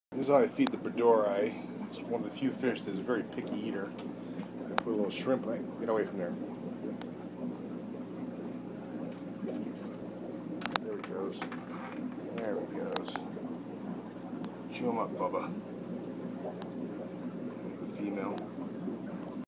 Today in the Fishroom ~ P. breidohri
Almost all of my fish eat the same stuff. Some more of one than the other. but none that are really picky. I have three exceptions. The male Breidohri, the male Freddie "yellow head" and the Rio Salto Carpinte. These three eat...almost exclusively...fresh seafood. shrimp and scallops.

Here's how I feed the male breidohri.  He recently lost the small tip of his dorsal fin to the female.